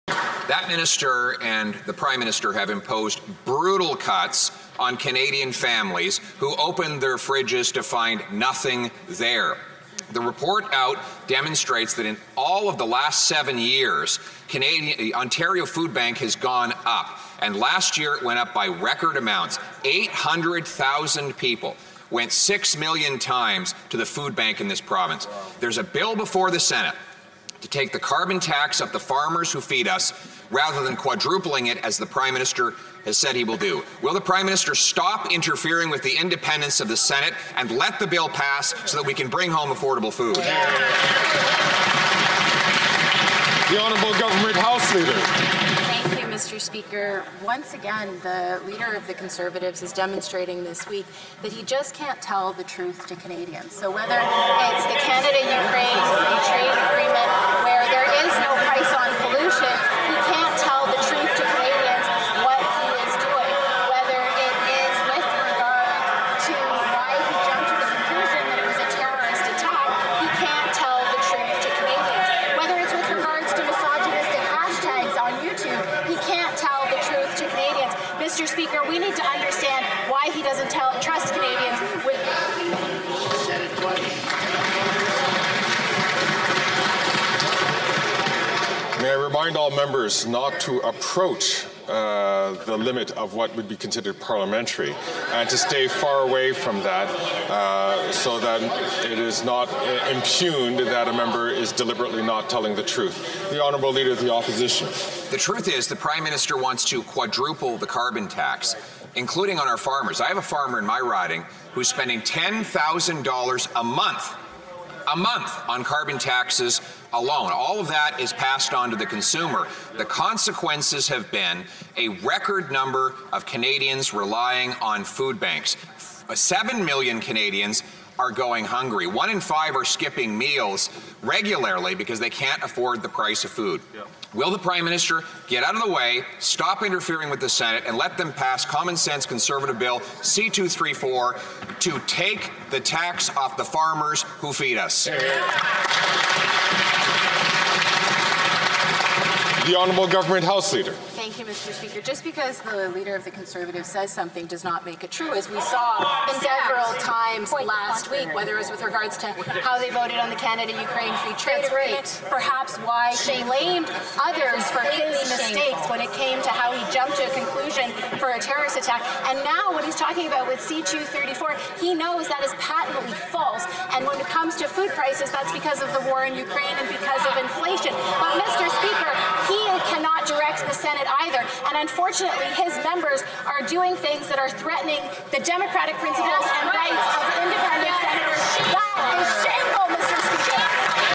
House Speaker Fergus pauses question period after tense exchange
House Speaker Greg Fergus had to pause question period after Minister Karina Gould accused Pierre Poilievre of being dishonest.